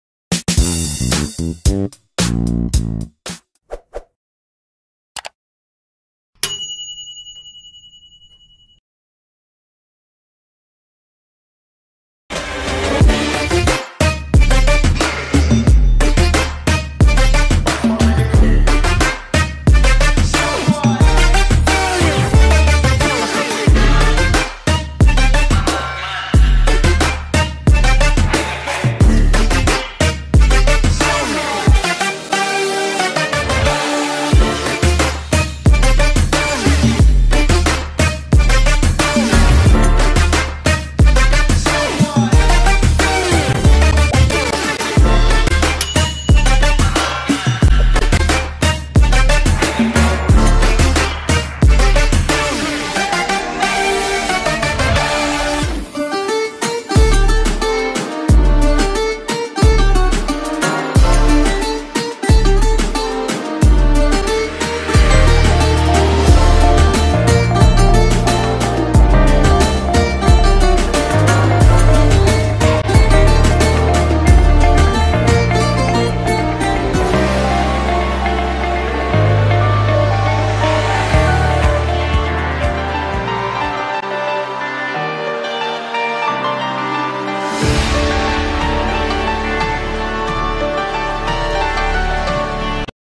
Video Iklan fashion, promosi pakaian mandi, baju tidur, baju mandi, promosi busana, wik wik dulu ah
Video Iklan fashion, promosi pakaian mandi, baju tidur, baju mandi, promosi busana, wik wik dulu ah. Video berikut adalah video company profile perusahaan digital. Nama perusahaan di video ini hanyalah fiktif belaka. Hanya sebagai contoh saja.